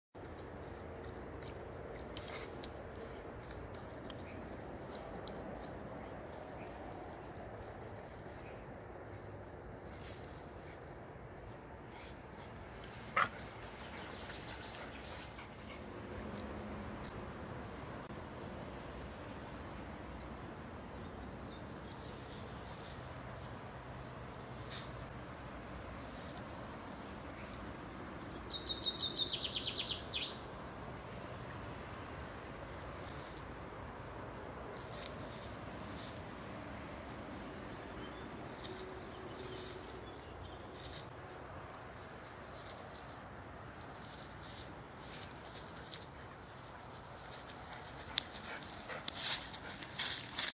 A vegetarian dog?
I though I had a dog but lately Huka has been eating a lot of grass, so now she is more like a sheep or a goat.